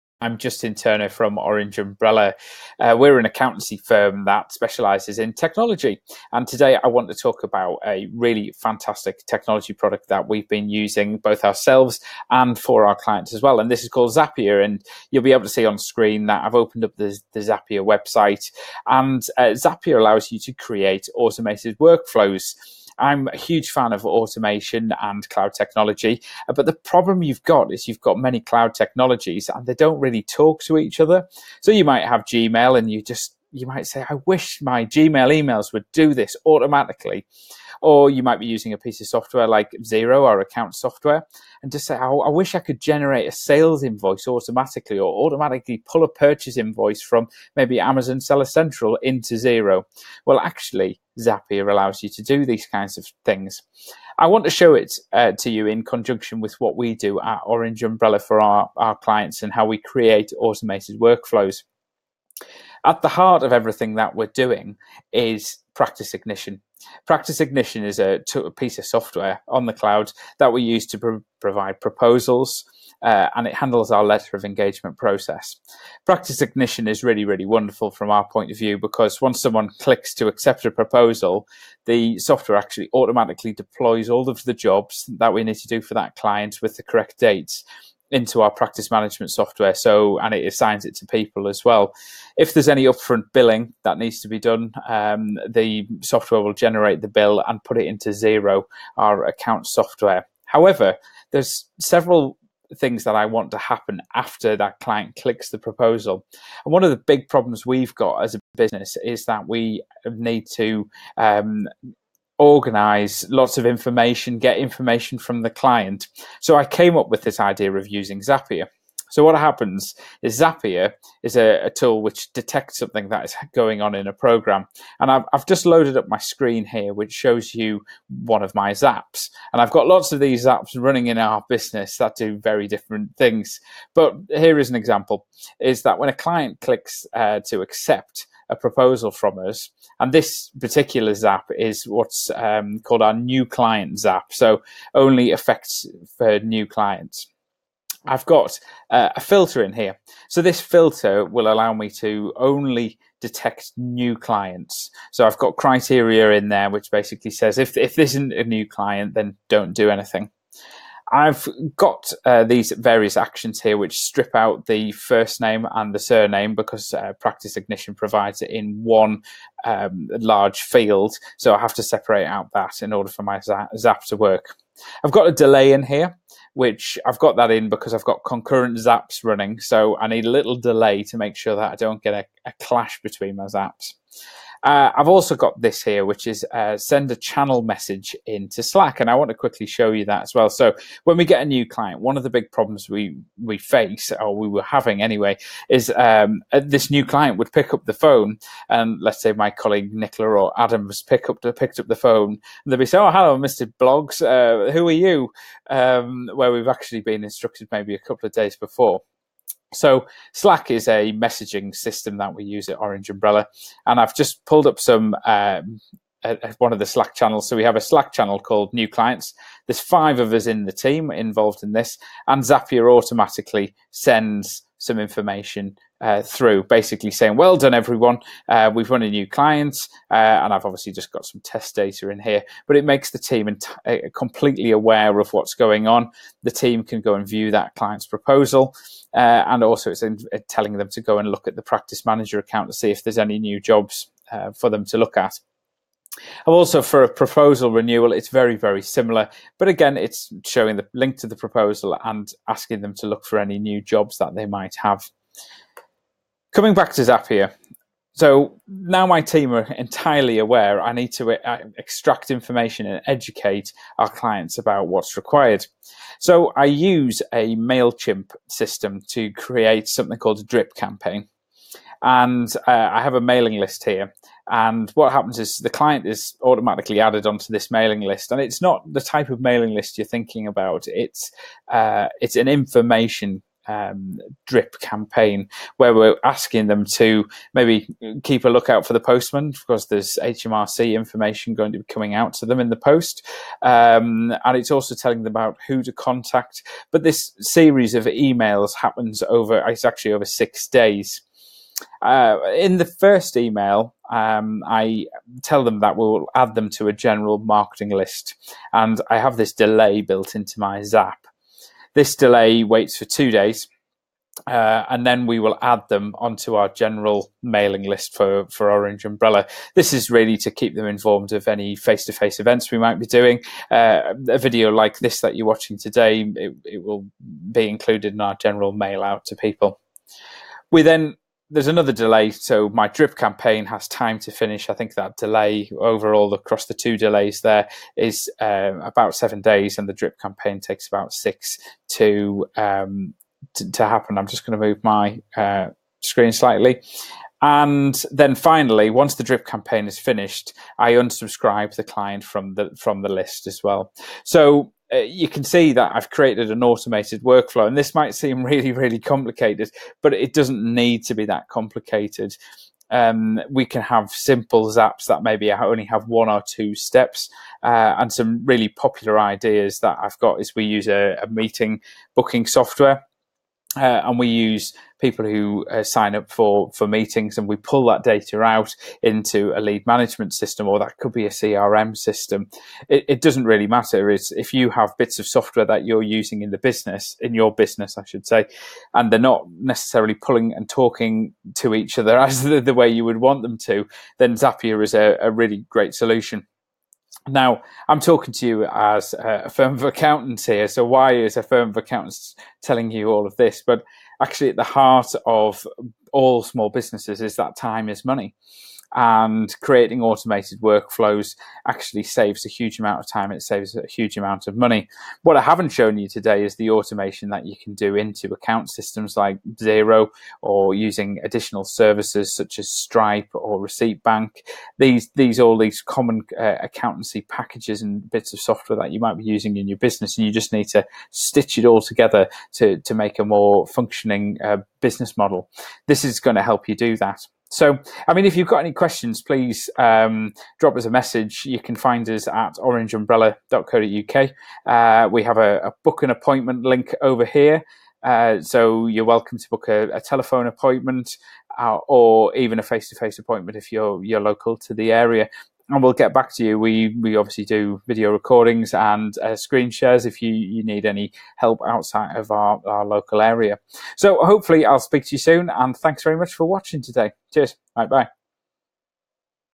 Create automated workflows using Zapier
We show how we use Zapier with Practice Ignition to create automated workflows. We integrate with apps like Mail Chimp and Google.